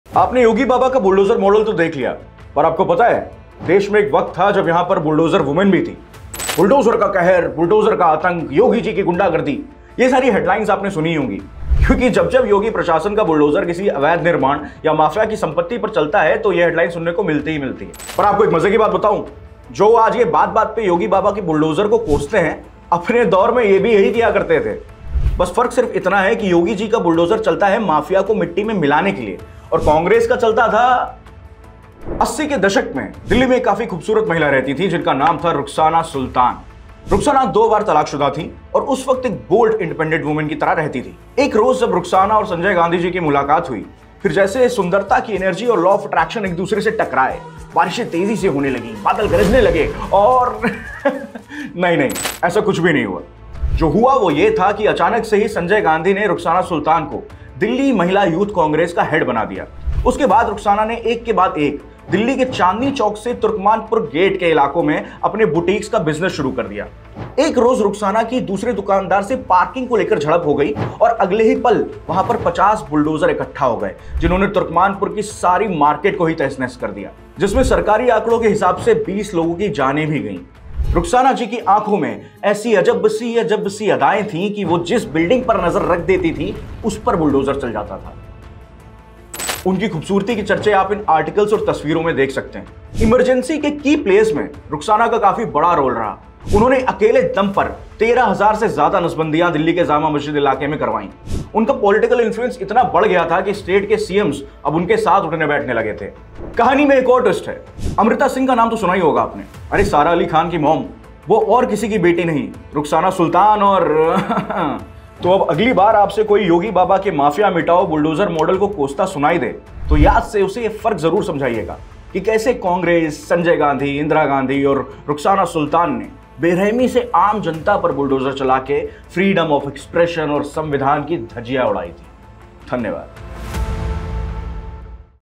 "BULLDOZER POLITICS" | HISTORY OF BULLDOZER IN INDIAN POLITICS | VINAYAK EXPLAINS
In this video, we respond to Dhruv Rathee's recent take on "Bulldozer Politics" in India. We delve into the historical context and evolution of bulldozer politics, revealing lesser-known facts and exposing the realities behind its use in Indian political strategies. Join us as we uncover the origins, implications, and controversies surrounding this powerful symbol in contemporary politics.